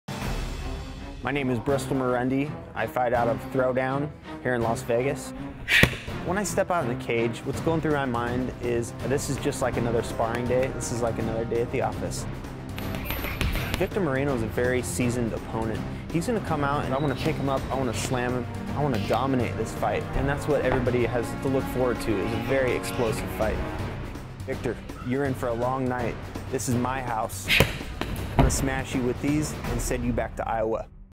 SCC 2 Fighter: Bristol Marunde
Bristol will fight Victor Moreno at SCC 2, August 20th at the Orleans Arena, Las Vegas.